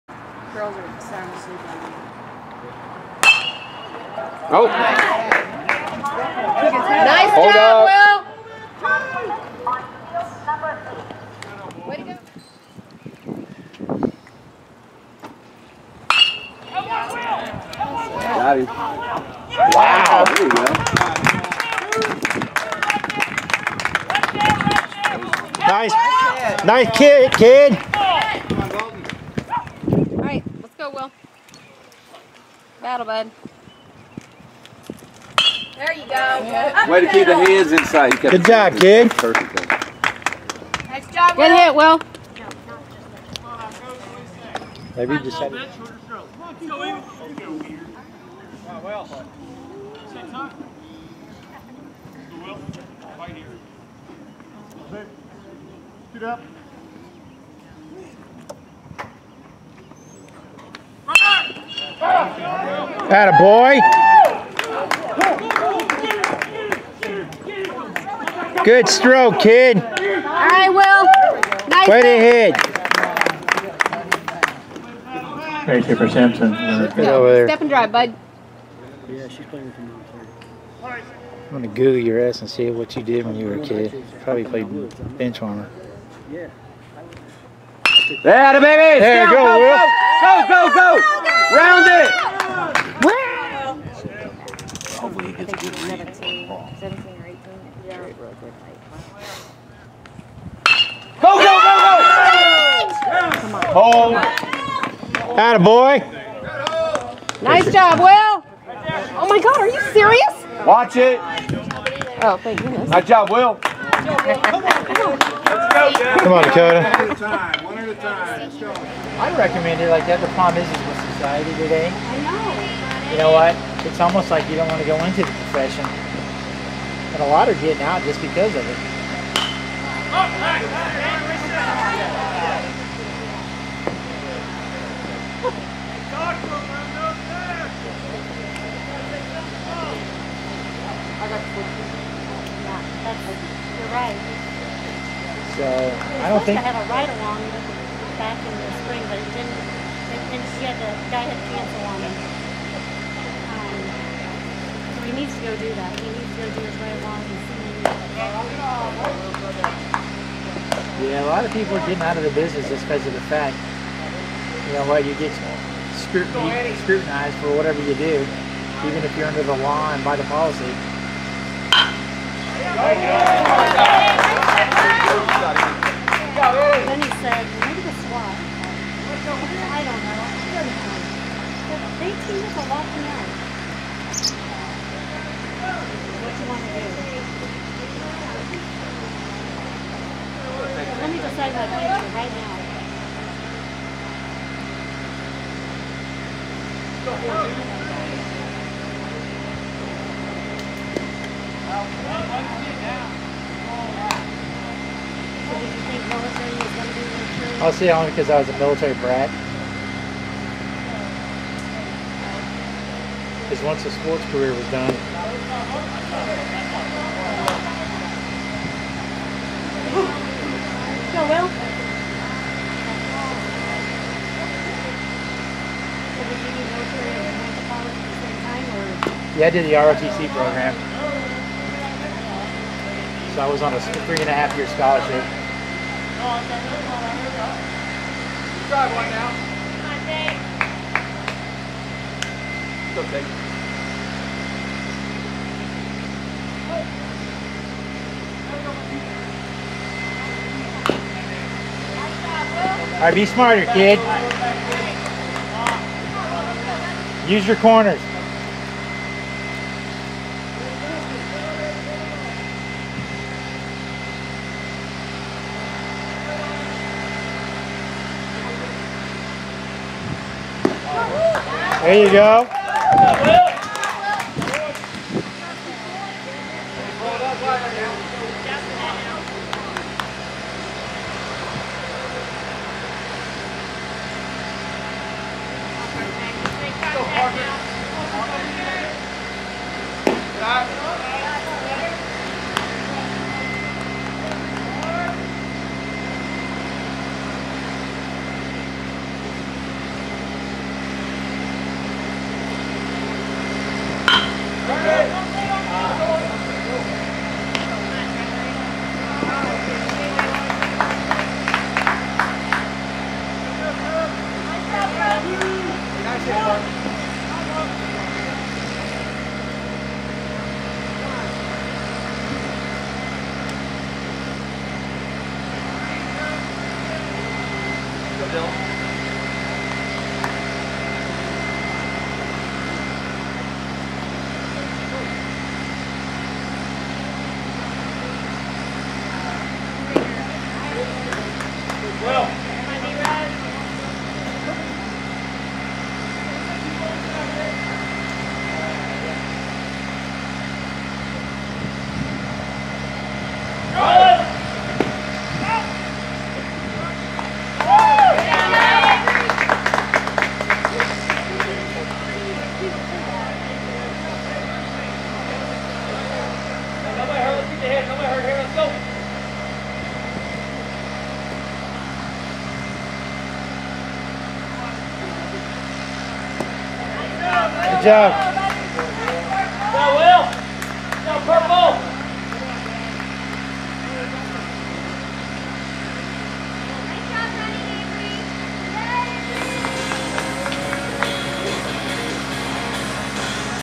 Will Simpson Recruitment Video
Hi I'm Will Simpson.  I've played varsity baseball at Oak Park High School for 3 years.  I'm 5'10 and 155 pounds.  My main positions are pitcher and middle infield.